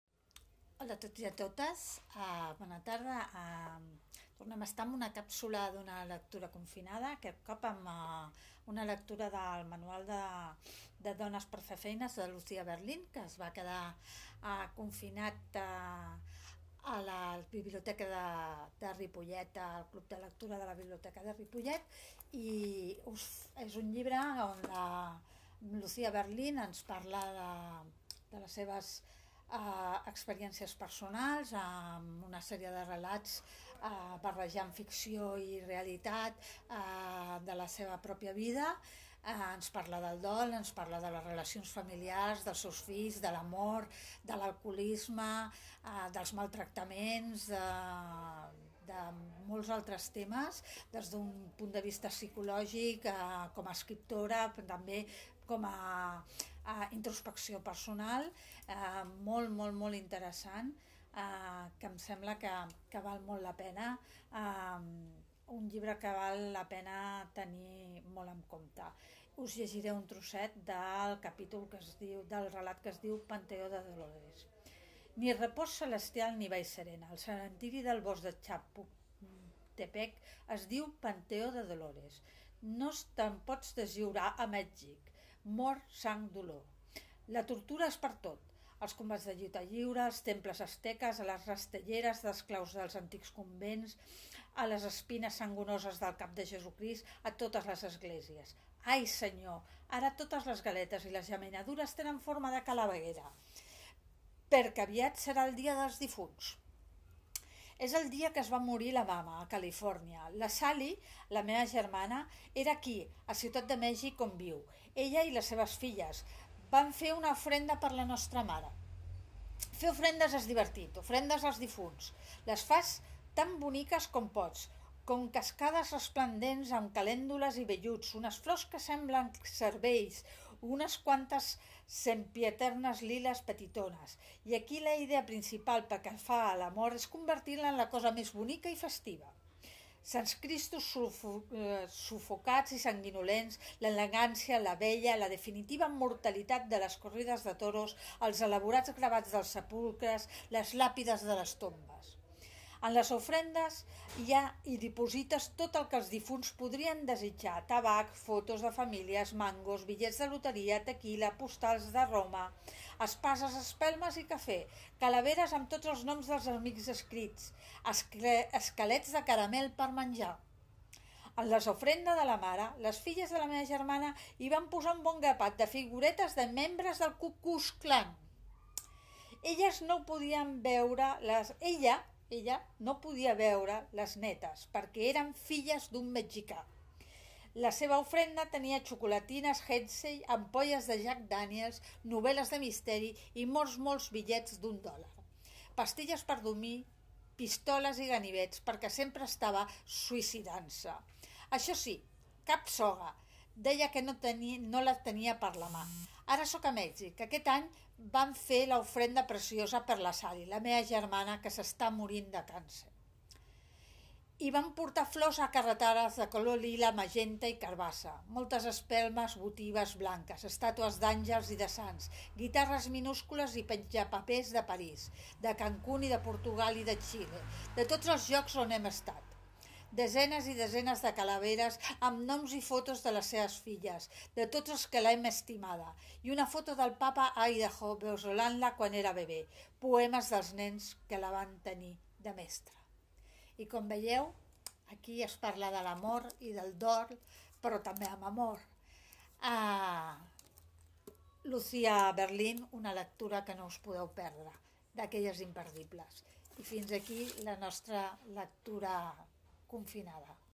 Càpsules de lectures confinades. Manual per a dones de fer feines. Lucia Berlin
Càpsules de lectures confinades. Manual per a dones de fer feines. Lucia Berlin. Viu la Lectura, Biblioteca de la Roca. CL d'Adults  Novel·la Biblioteca de Ripollet.  Lucia Berlín ens parla del dol, de l'amor, de la vida, de la familia, dels fills, a través de les seves experiències personals.

Capsulesdelecturesconfinades Serveis Educatius Secc. Alícia Gili Abad